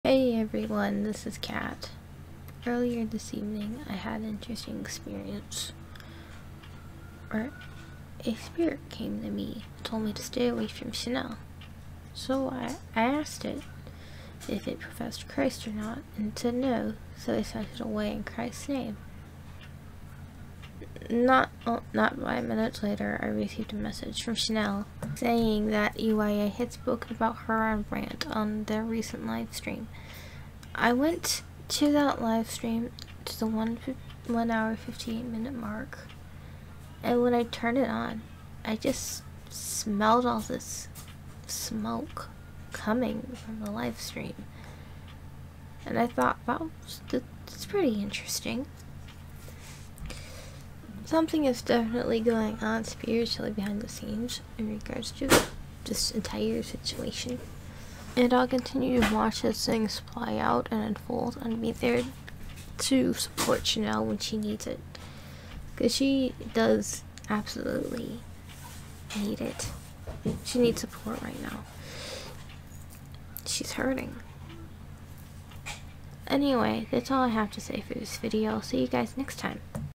Interesting events..
Strange things surrounding the Chanel and Brant situation..